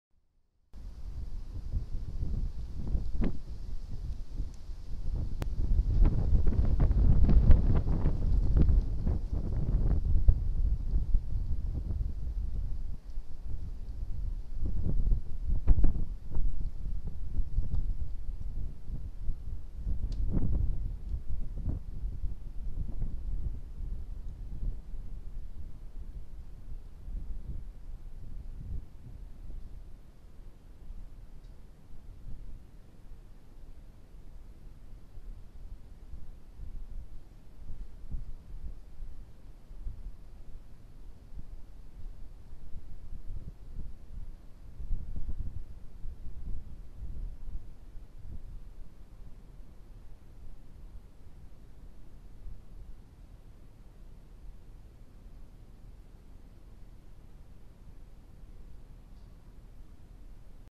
pine marten - glaring at fox
pine marten male intensely glaring at a fox just behind the camera.